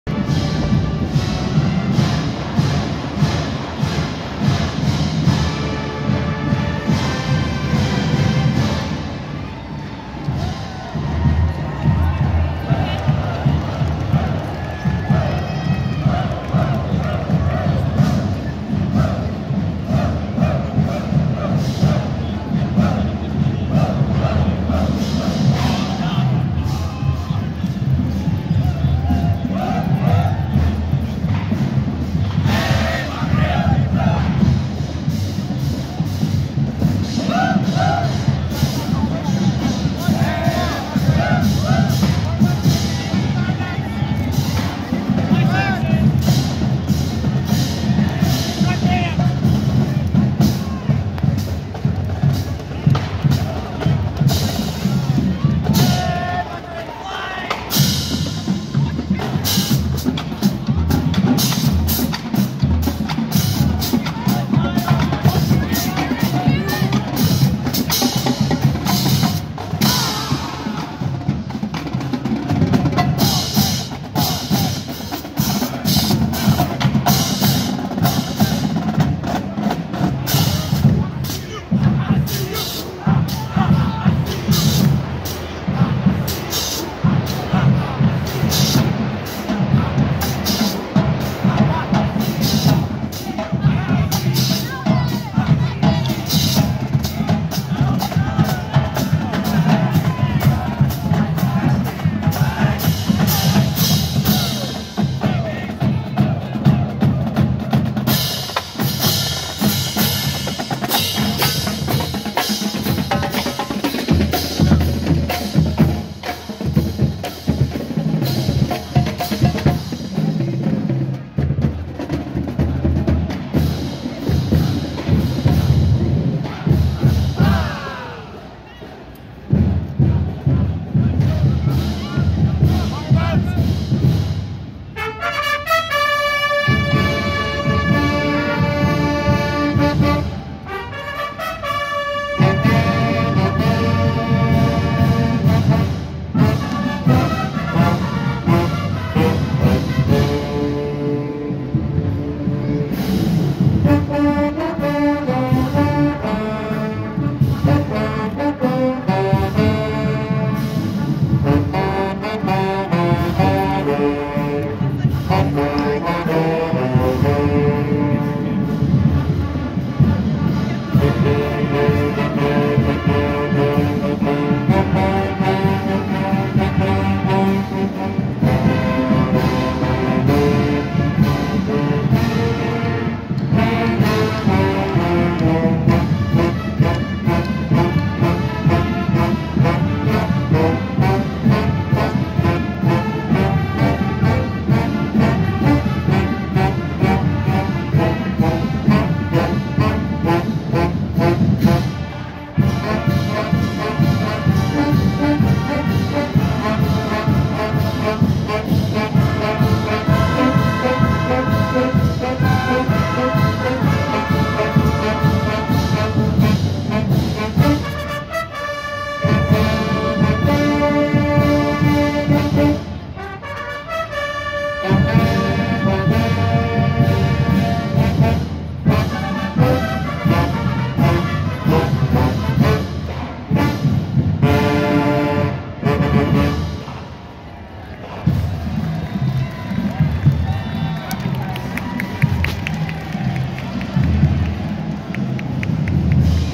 Chinese New Year's Parade 2020 San Francisco
Cal Poly Marching Band